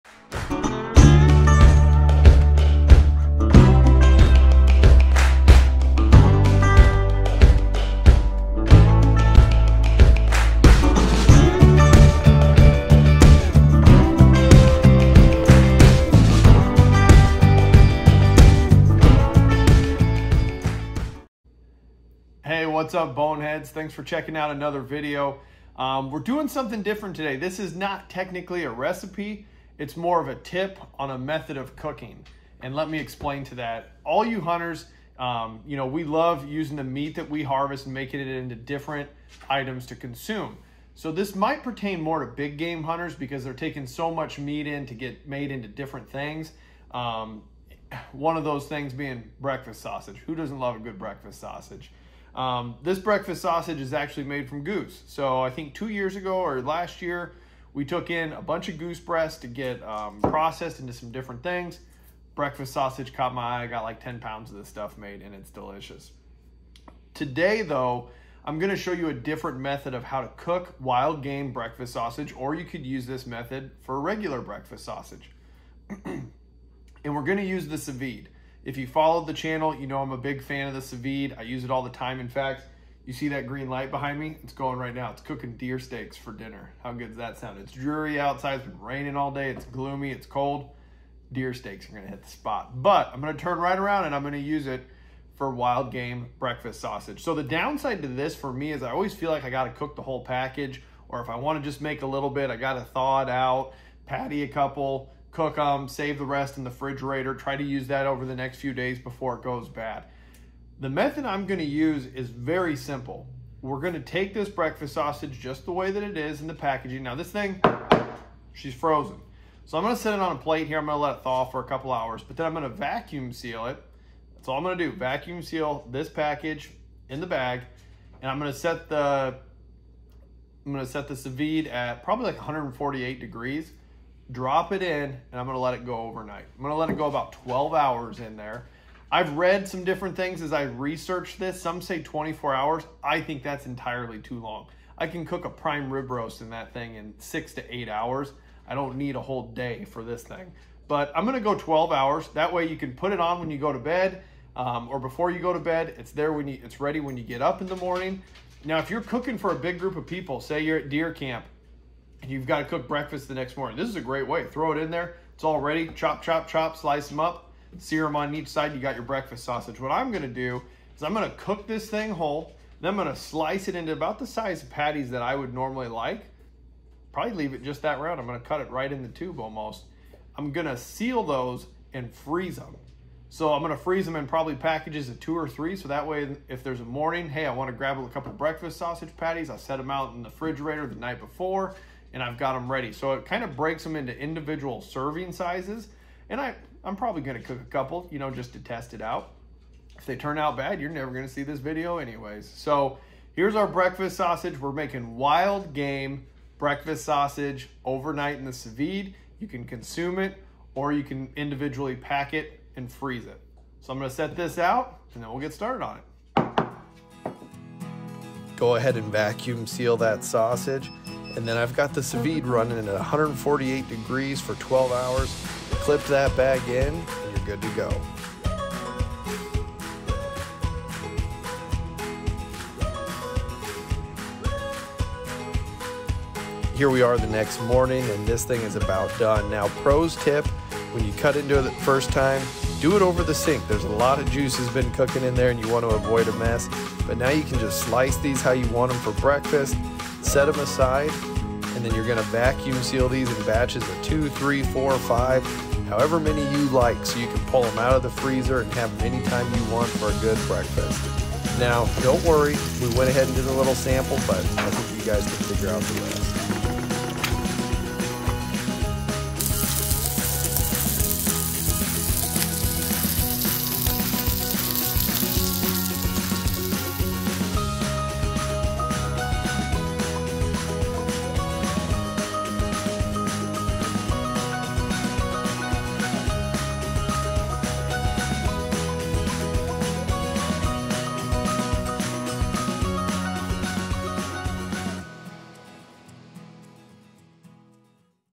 Wild Game Breakfast Sausage w/ Sous Vide
Here is an easy way to cook an entire package of wild game breakfast sausage. The best part is you can eat it immediately, or package/freeze it in smaller batches! Simple as putting it in the sous vide, cut it up the next morning and you're good to go!